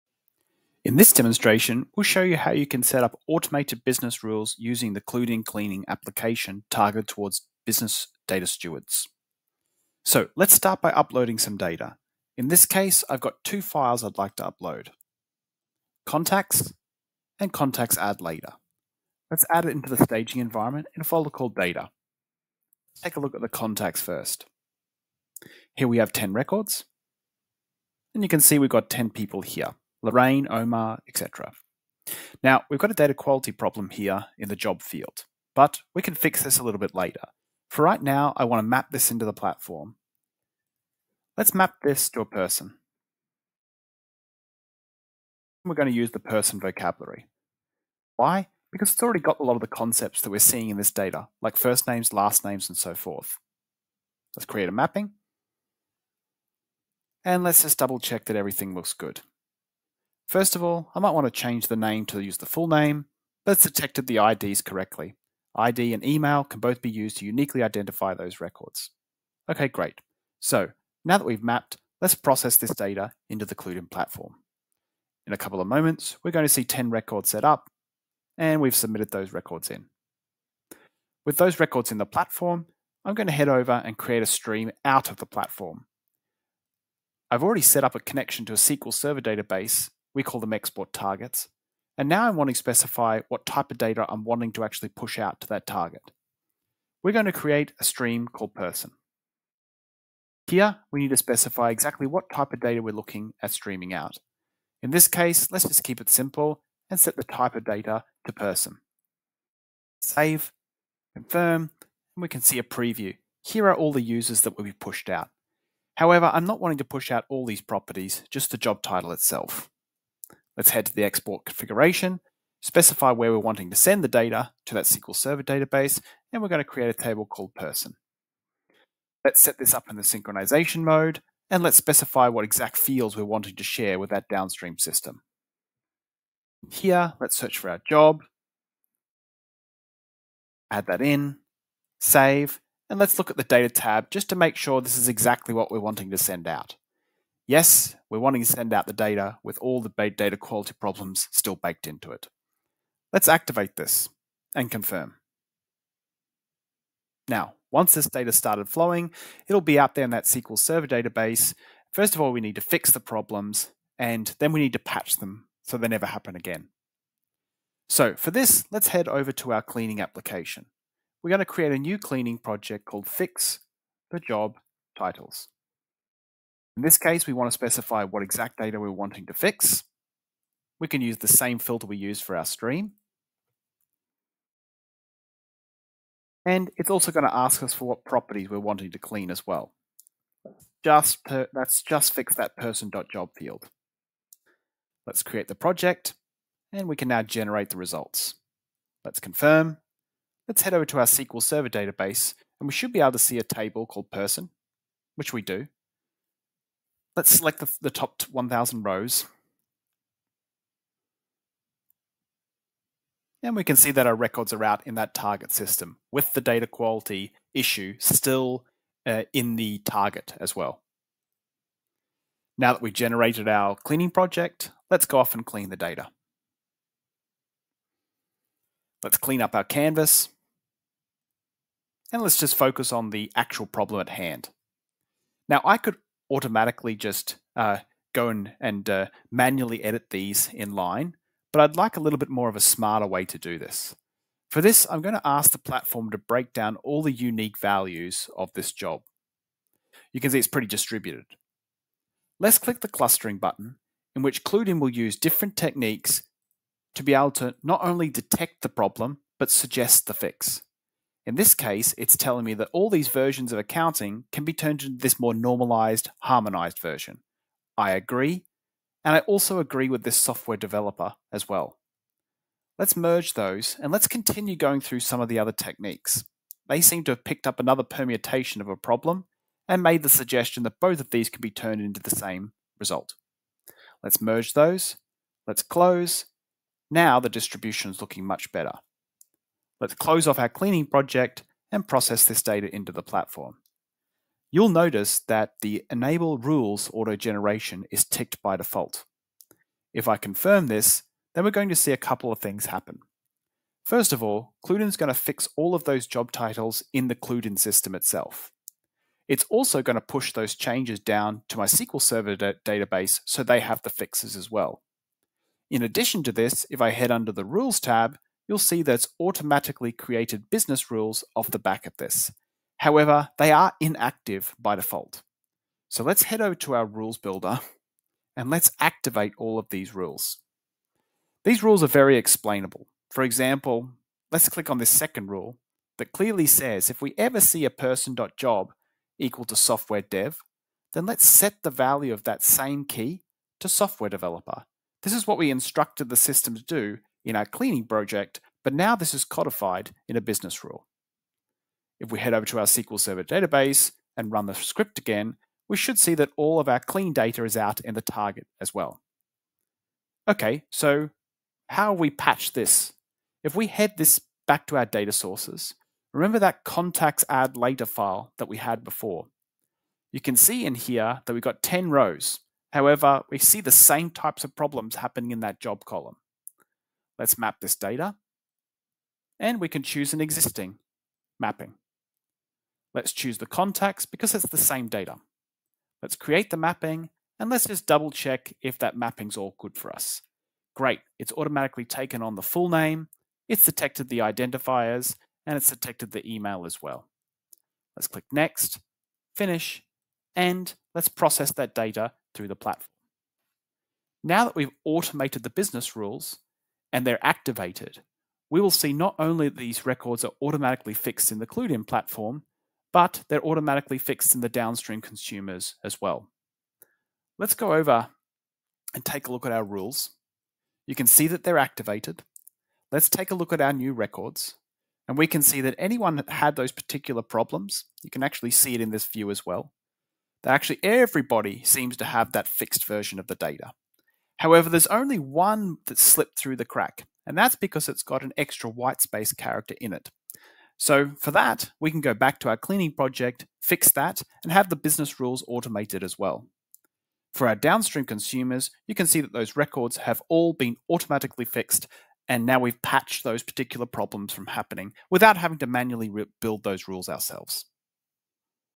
Build automated business rules with CluedIn (Microsoft Master Data Management with Azure OpenAI)
In this quick demo, we show you how the CluedIn Clean application allows you to set up and implement automated business rules.